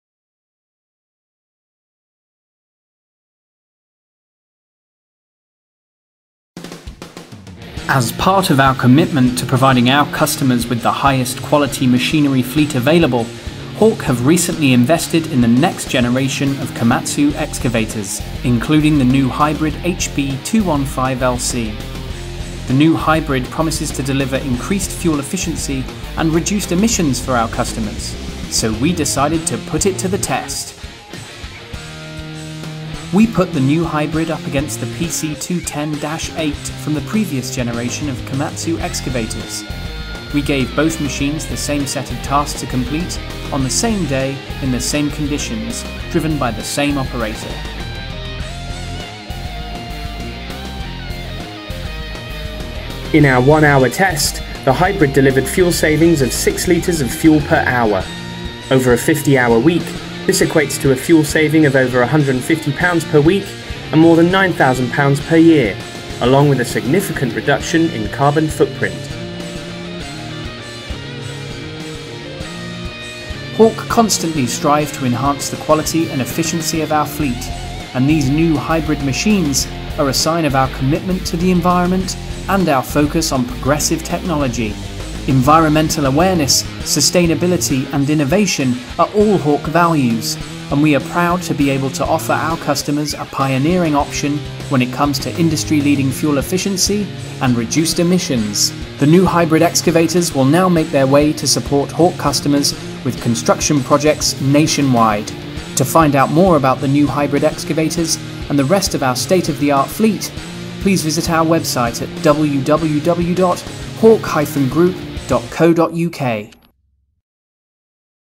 Komatsu Hybrid Excavator test
New Komatsu Hybrid HB215LC excavators have recently joined the Hawk plant hire fleet. In this video we put them to the test, demonstrating a fuel saving over £9000 per year.